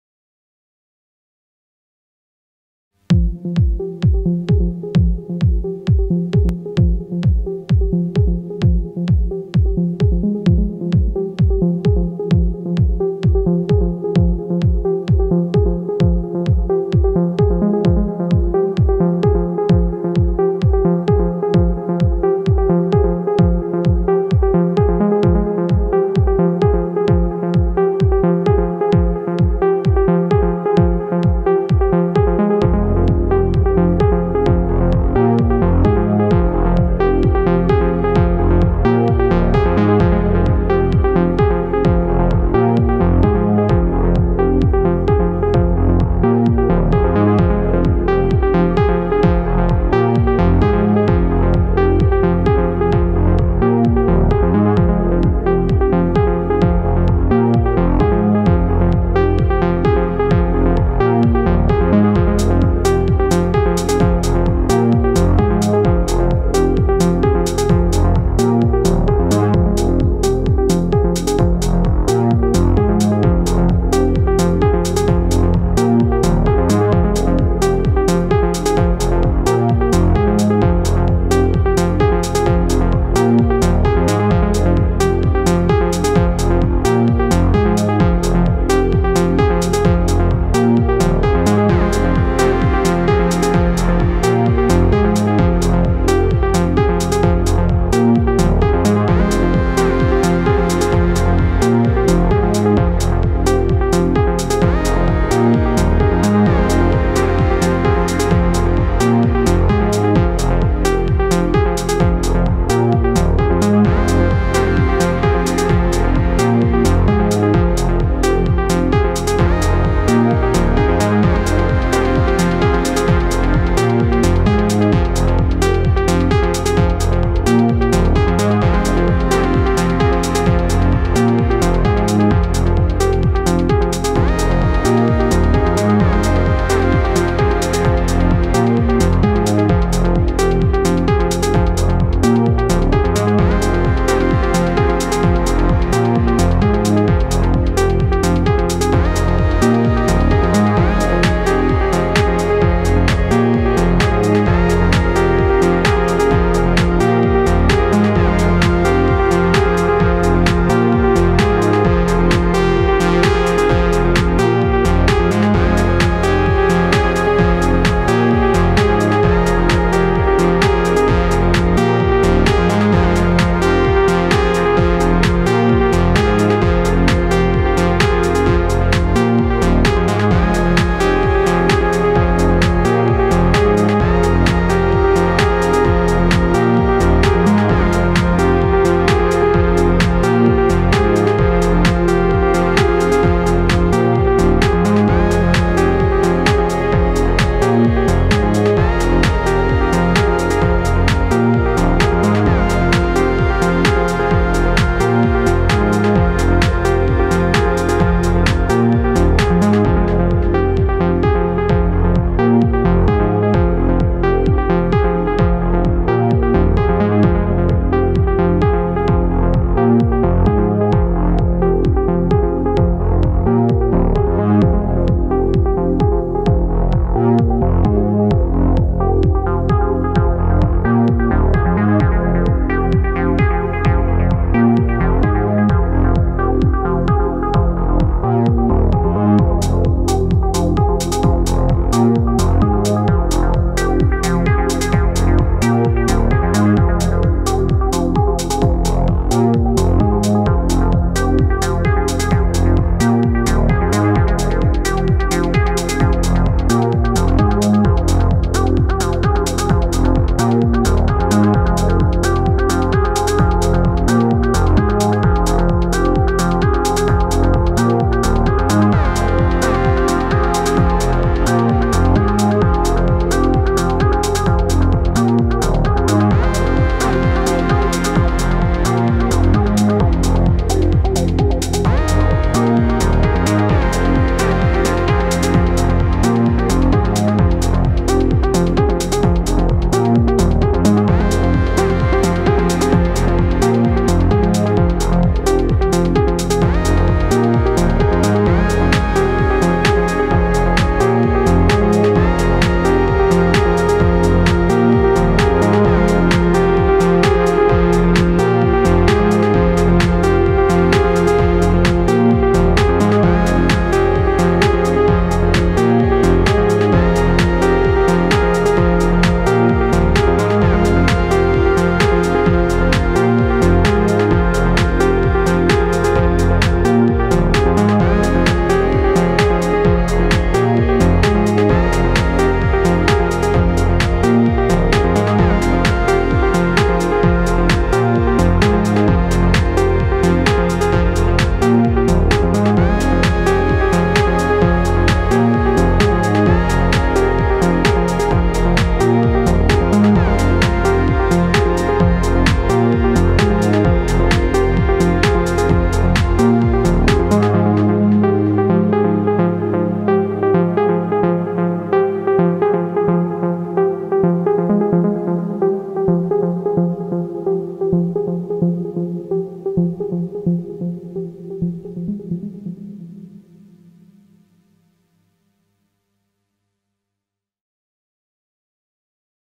Elektron Model: Samples || Dawless Techno Jam || Neutron || Crave || Skulpt
Late night techno jam with hardware synths. All sounds are coming from hardware synths. Only vst FX ( reverb, delay, compression) was added.
Model Samples as main control unit sending midi to Crave, Neutron and Skulpt.
Lead - behringer crave
Bass - behringer neutron
Pads - modal skulpt
Drums - elektron model:samples